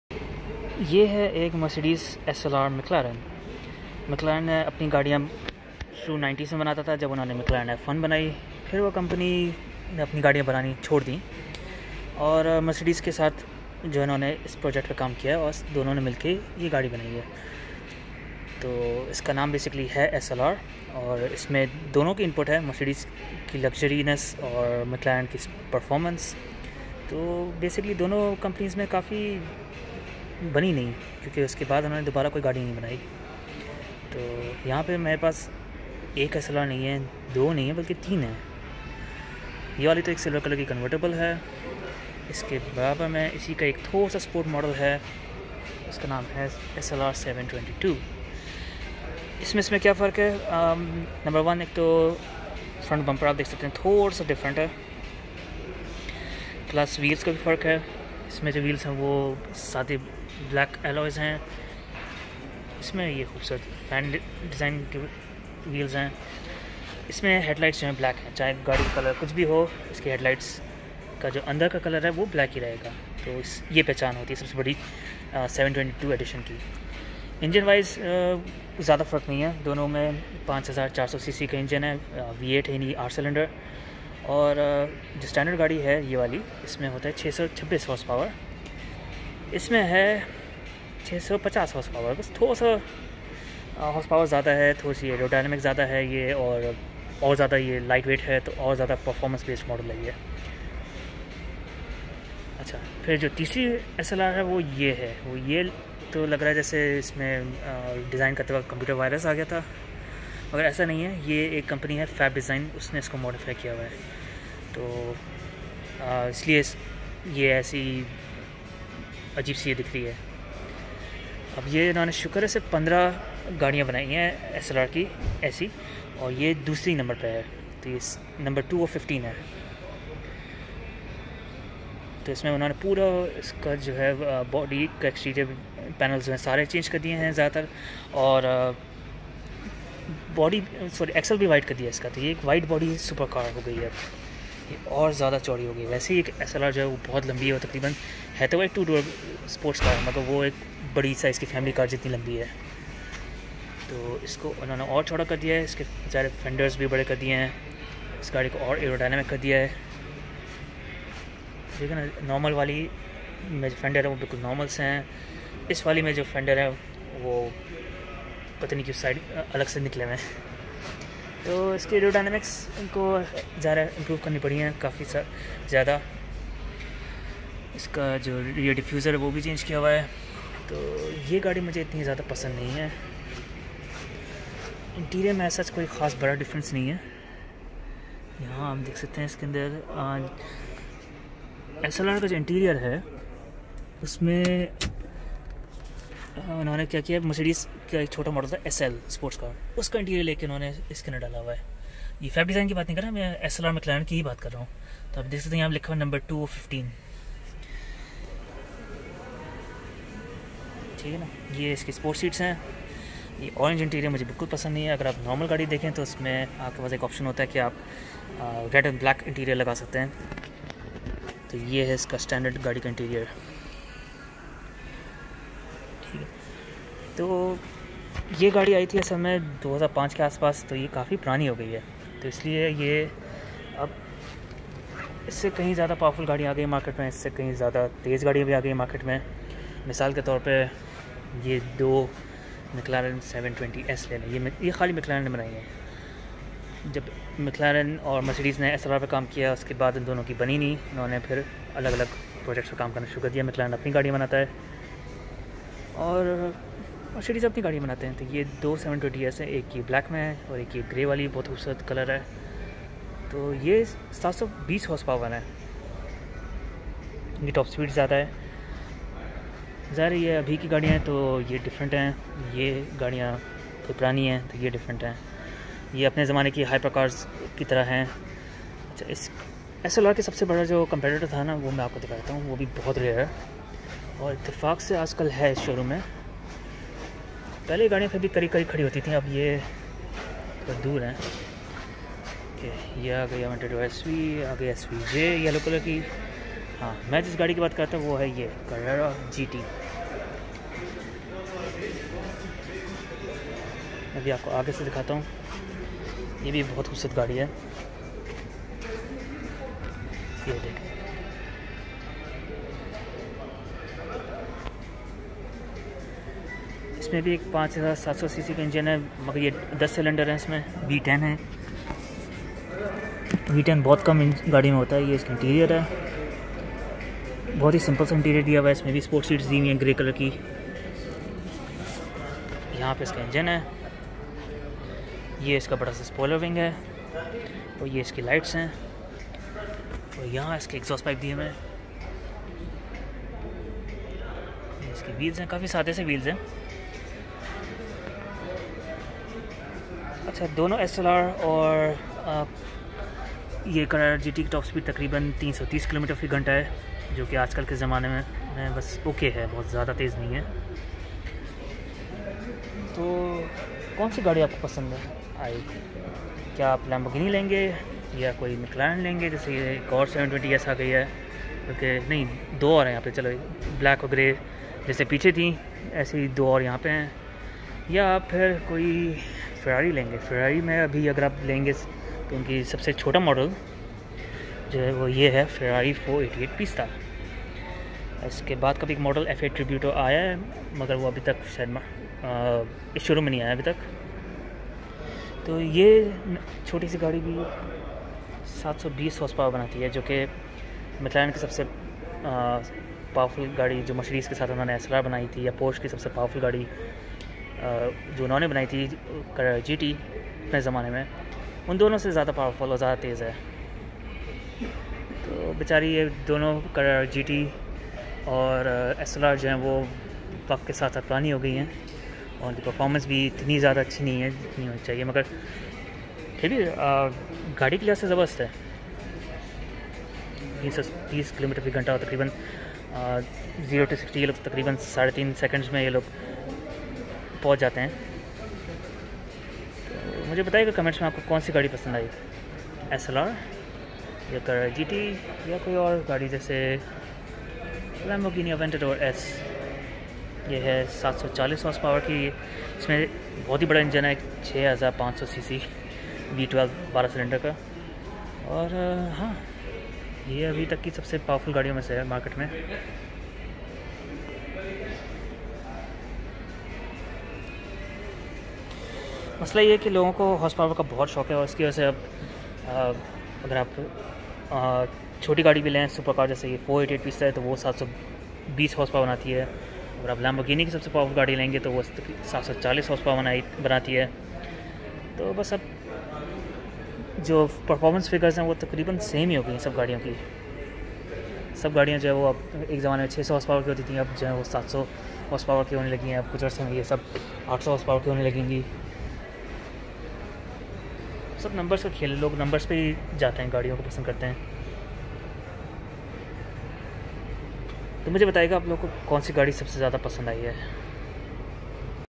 Mercedes SLR McLaren vs Porsche Carrera GT review - Urdu
Mercedes Benz SLR McLaren
5400cc V8 aluminum hand made engine, 626 horsepower

Porsche Carrera GT
5,700cc V10 engine, 612 horsepower
Top speed: 330 km/h (205 mph)
only 1270 cars made